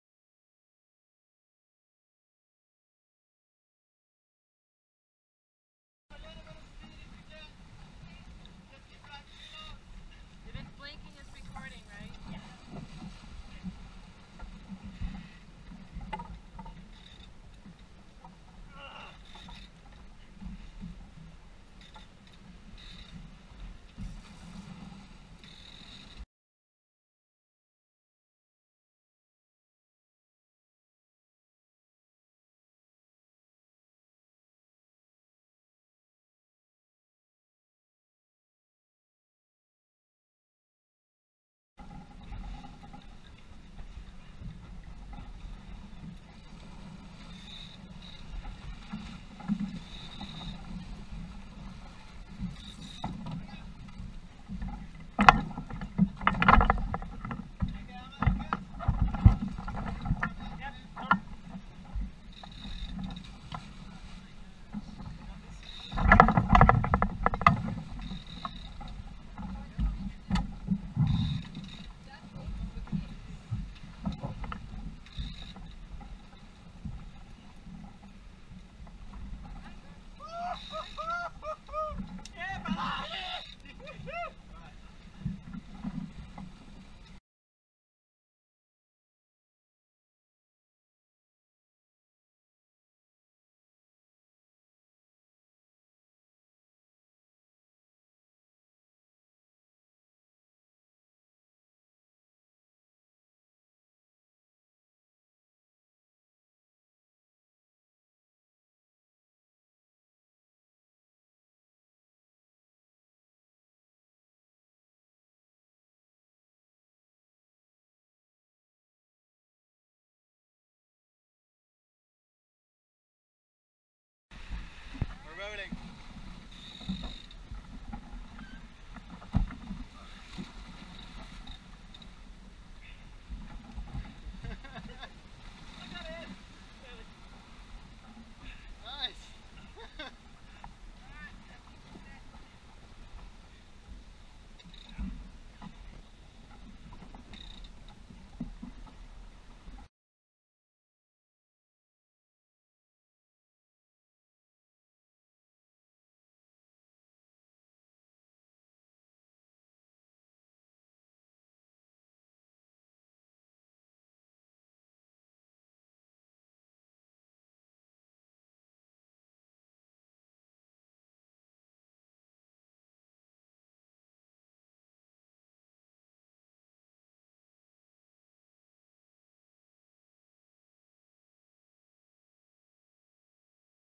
Yellow Fin - The Monster From The Deep!
A small fish I caught - Somewhere in the South Pacific